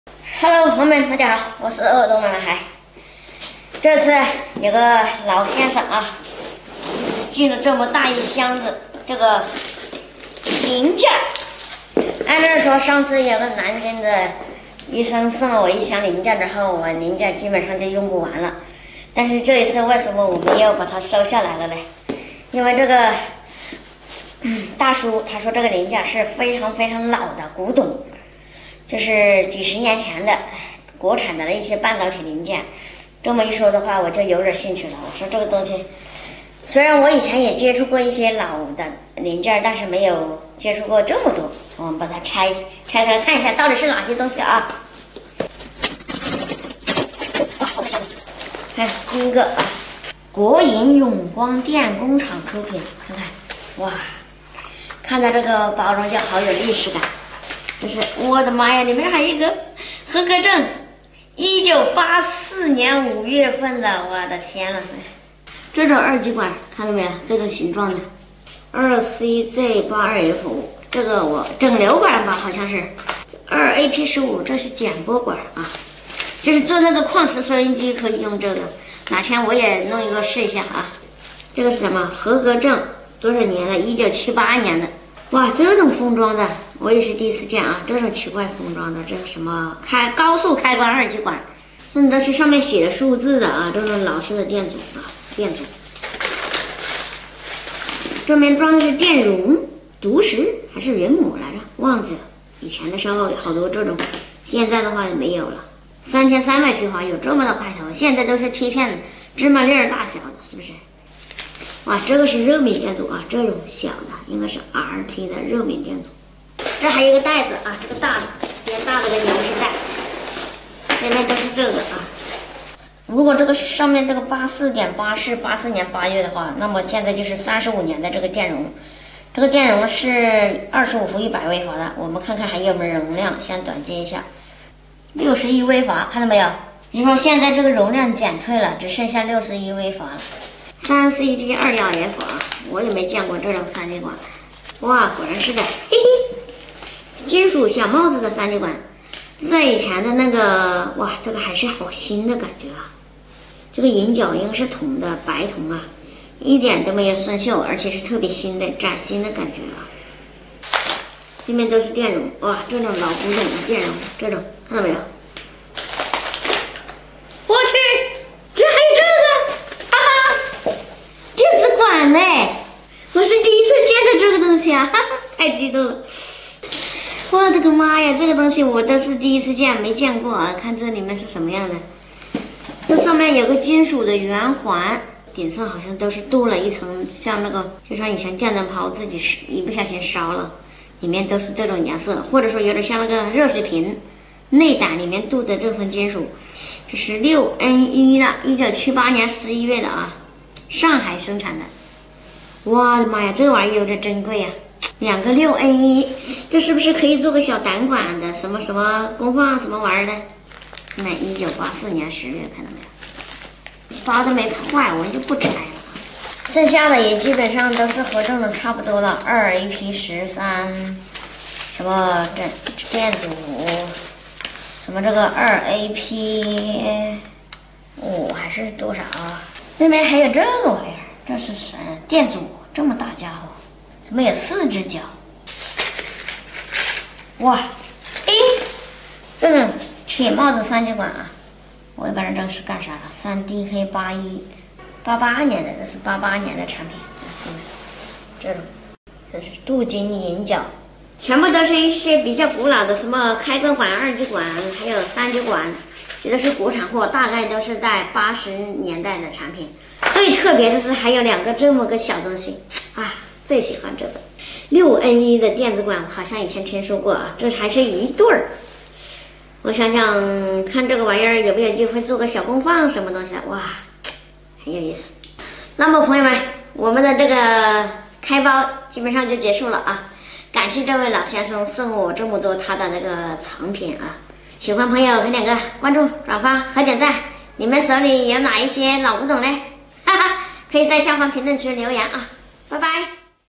网友寄来一个大箱子，都是30年前的珍藏，拆到啥把老男孩激动得
鄂东老男孩 【鄂东老男孩】 鄂东老男孩，vlog，网友大叔寄来一个大包裹，都是大叔的珍藏，拆到啥老男孩激动了一把，摆放了一大桌，这些宝贝你都见过吗？